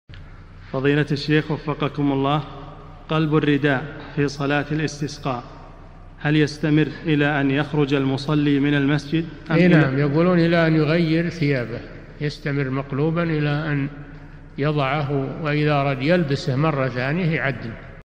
[575 -1024] قلب الرداء في صلاة الاستسقاء هل يستمر إلى أن يخرج المصلي من المسجد؟ - الشيخ صالح الفوزان
-[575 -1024] السؤال: قلب الرداء في صلاة الاستسقاء هل يستمر إلى أن يخرج المصلي من المسجد؟
الجواب: إي نعم يقولون إلى أن يغير ثيابه، يستمر مقلوبا إلى أن يضعه وإذا أراد أن يلبسه مرة ثانية يعدله.
للاشتراك في قناة الشيخ صالح الفوزان
سؤال وجواب من شرح الفتوى الحموية ( 1024 ) مقطع

للاستفسار والإقتراحات والدعم
رئيس الفريق
لمشروع كبار العلماء